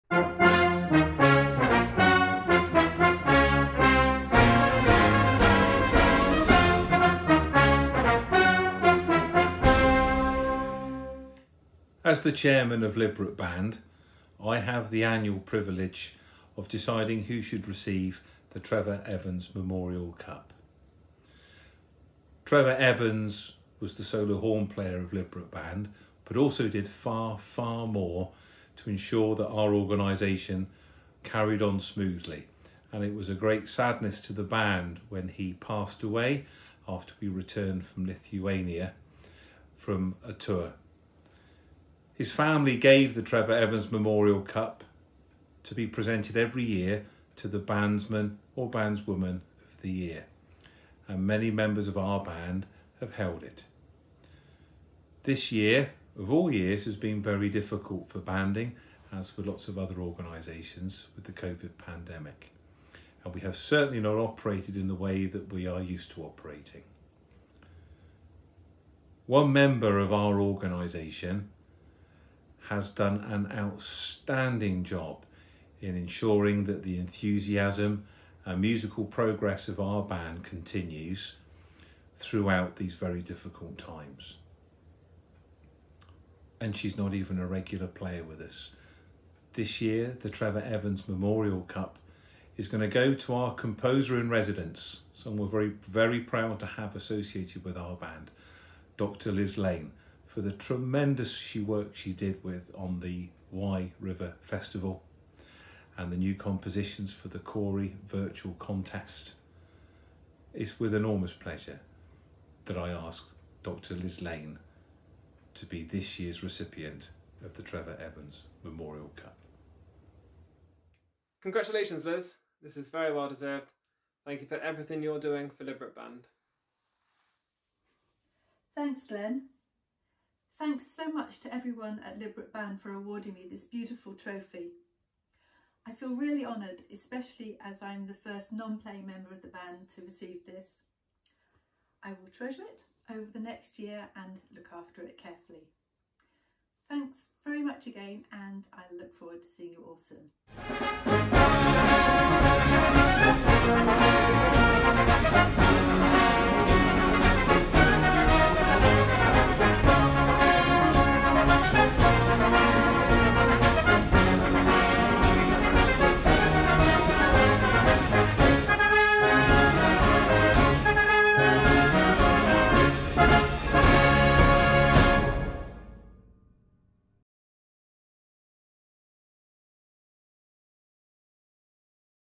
Bandsperson of the Year 2020
The Trefor Evans Memorial Cup is awarded by band chairman, Trevor Moss, at our annual Christmas Concert.  However, due to the COVID-19 pandemic we were unable to do so.
2020's recipient is the first non playing member of the band to receive the reward; in part as recognition for their had work on the Wye Valley River Festival last summer and our Cory Online Brass Band Championships entry.
Watch the video below to find our who the winner is, and for the virtual trophy presentation!